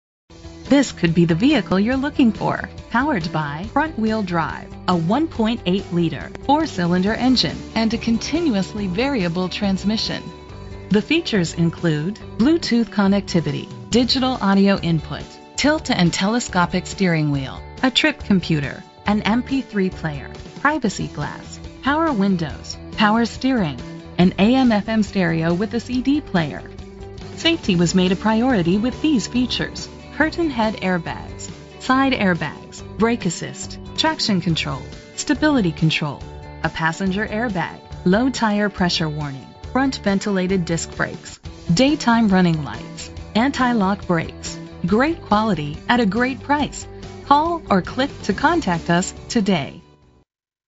2015 Toyota Corolla - Escondido CA
Year: 2015
Make: Toyota
Model: Corolla
Trim: LE
Engine: 1.8 liter inline 4 cylinder 16 valve MPFI DOHC
Transmission: Automatic CVT
Color: Brown Sugar Metallic
Mileage: 1
Address:
231 E Lincoln Pkwy